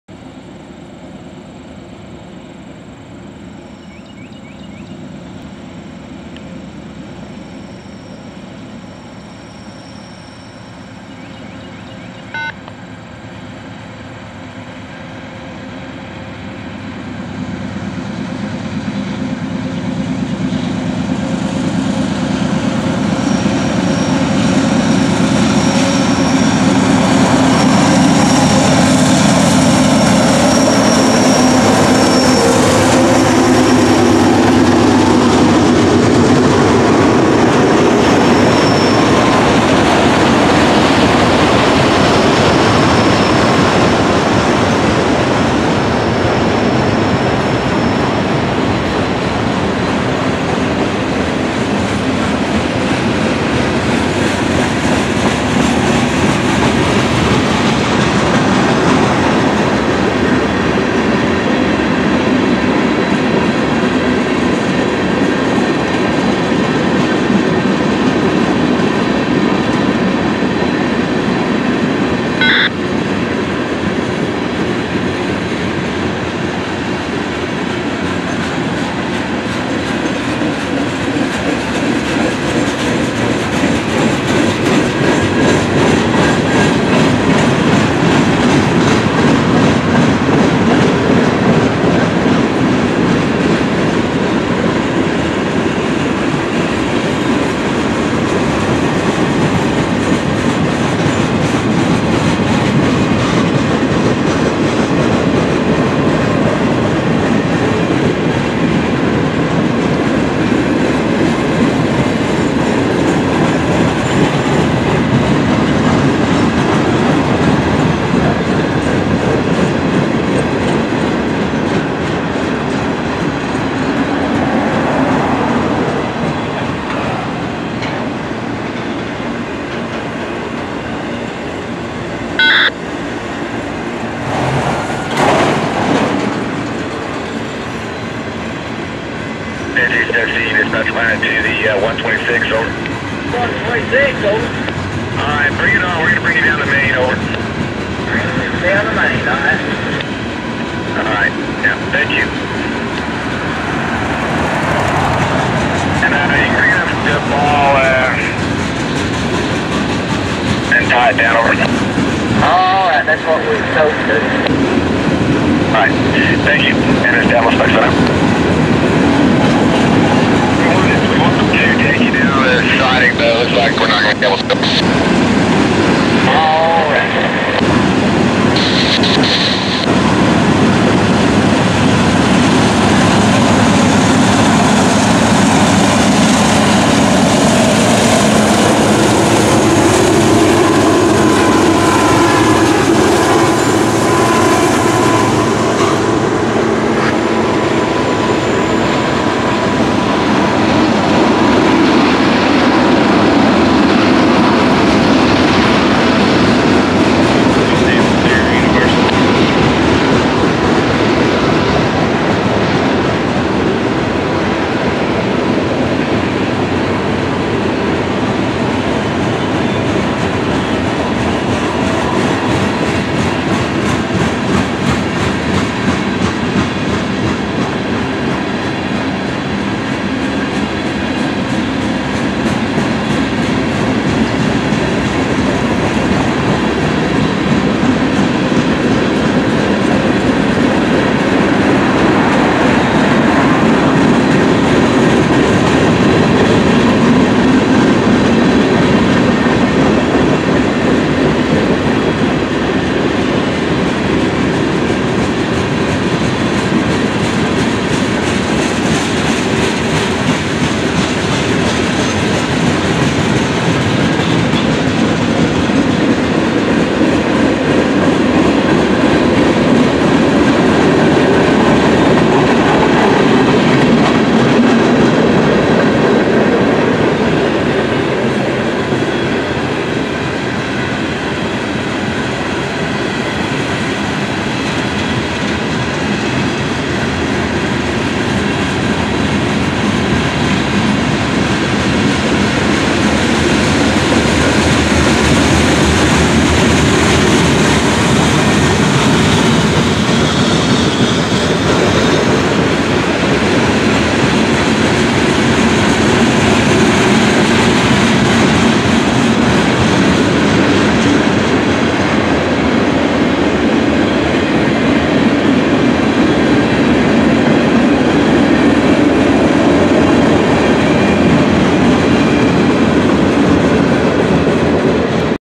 Train 126 10/29/22. Bristol, TN. Has 3 UP Engine's.
Eastbound! Train 126 10/29/22 on Saturday in Bristol, Tennessee. Has 3 UP Engine's.